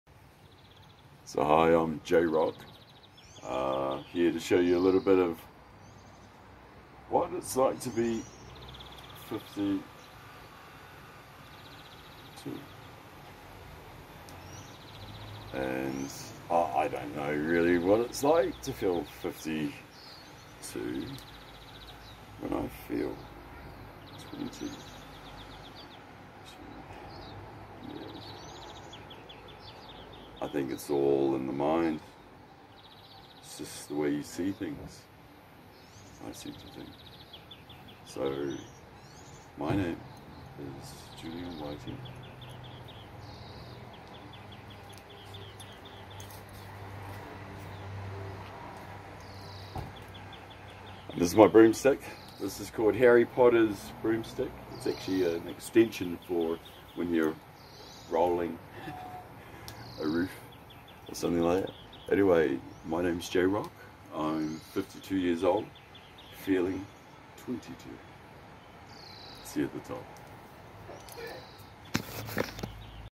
Being 52 years, living more 22.
I answered a question that I often get asked... "What's it feel like to live at 50+ plus... answer; I DON'T FEEL 50+. because of the little things made easy.
I do issollaition exercises with a pole, and refresh myself by walking upright in posture or strolling briskly, at least 20 - 40 minutes and up to 3x times per day. Excellent to get Vitamin D form sun light and pump MASS AIR into your lungs.